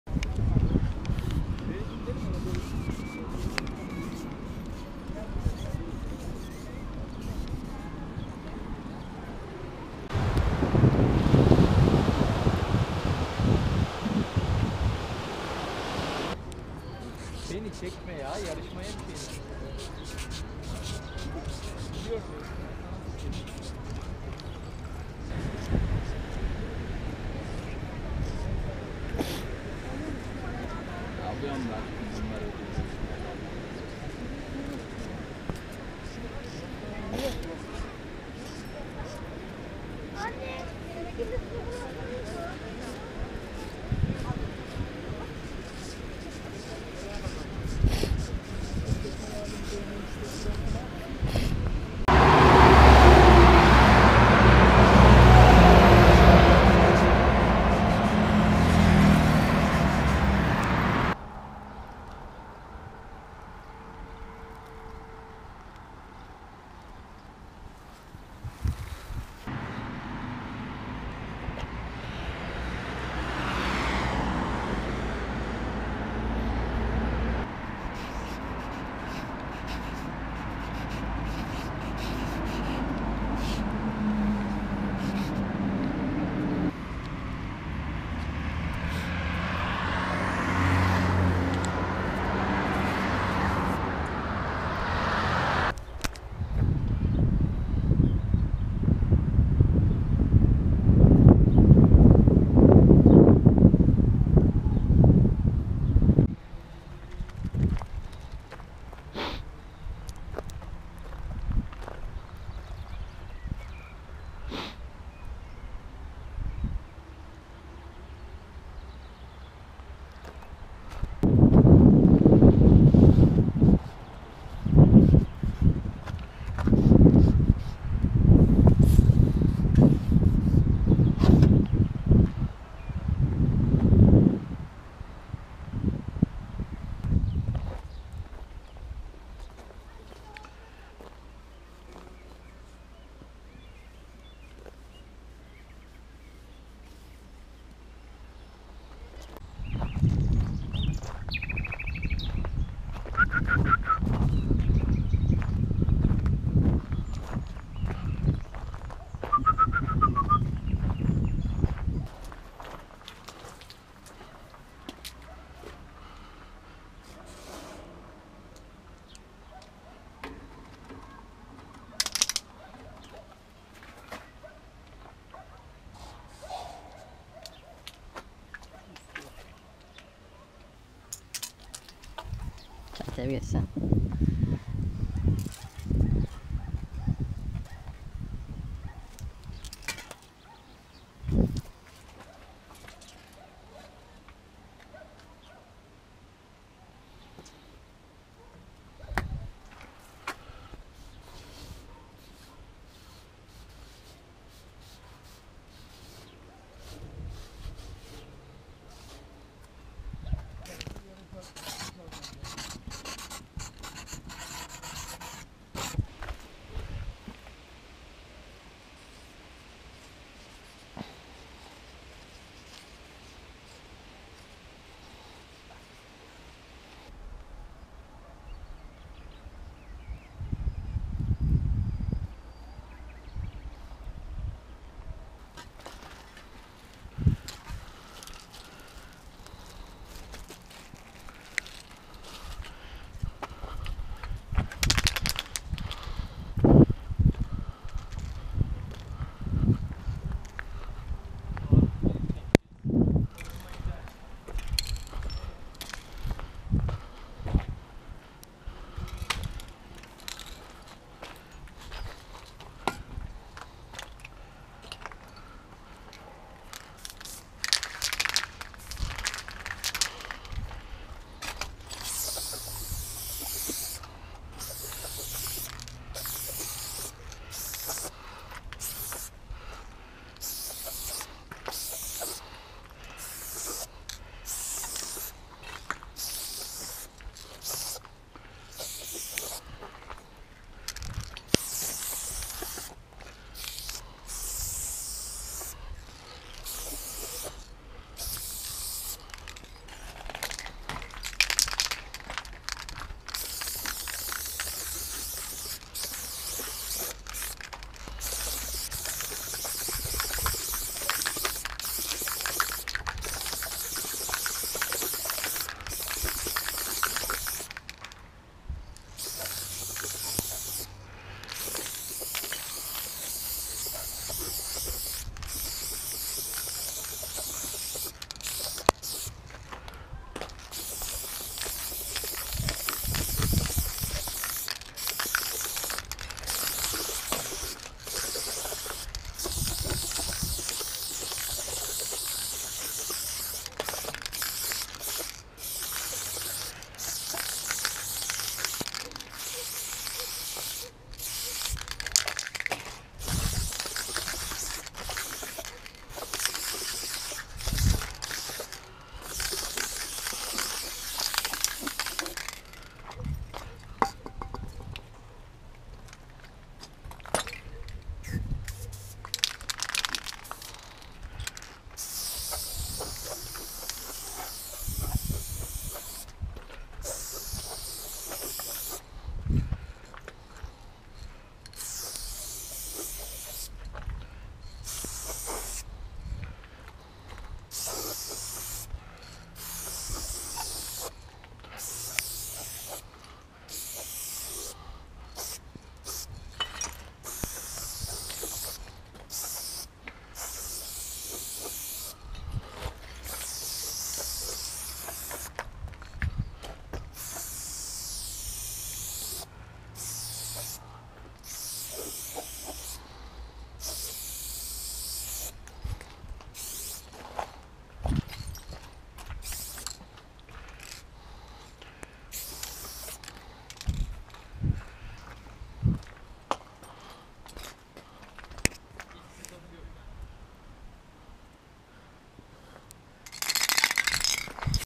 Graffiti Trip 2 - ThrowUp X Tagging | Gopher
On an ordinary day, I'm walking while doing graffiti.